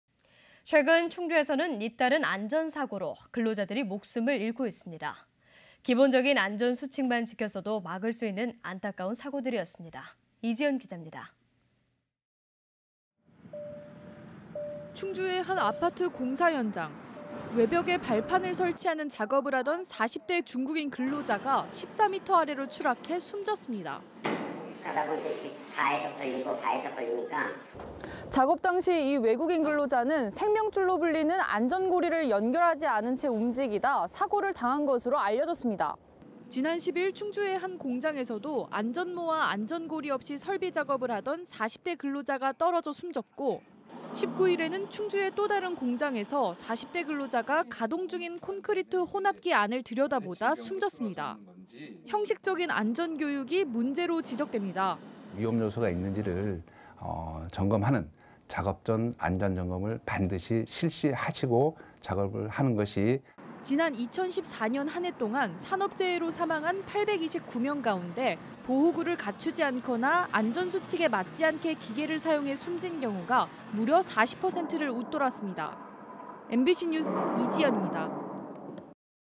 MBC충북 NEWS 161124 안전불감증' 대체 언제까지
◀ＡＮＣ▶
 최근 충주에서 잇따른 안전 사고로
근로자들이 목숨을 잃고 있습니다.
 기본적인 안전 수칙만 지켰어도
막을 수 있는 안타까운 사고들이었습니다.
 이지현 기자입니다.
           ◀ＥＮＤ▶

 충주의 한 아파트 공사 현장.

 외벽에 발판을 설치하는 작업을 하던
40대 중국인 근로자가 14m 아래로 추락해
숨졌습니다.

           ◀ＳＹＮ▶ 건설업체 관계자(변조)
"기둥에 걸려"

(S/U) 작업 당시 외국인 근로자는
생명줄로 불리는 안전고리를 연결하지 않은 채
움직이다 사고를 당한 것으로 알려졌습니다.

 지난 10일 충주의 한 공장에서도
안전모와 안전고리 없이 설비 작업을 하던
40대 근로자가 떨어져 숨졌고,

 19일에는 충주의 또다른 공장에서
40대 근로자가 가동 중인 콘크리트
혼합기 안을 들여다보다 숨졌습니다.

 형식적인 안전 교육이 문제로 지적됩니다.

           ◀ＩＮＴ▶ 권기섭 과장
"형식적인 교육 이상의 점검 필요"

 지난 2014년 한 해 동안
산업 재해로 사망한 829명 가운데
보호구를 갖추지 않거나 안전수칙에
맞지 않게 기계를 사용해 숨진 경우가
무려 40%를 웃돌았습니다.
MBC 뉴스 이지현입니다.//영상취재 양태욱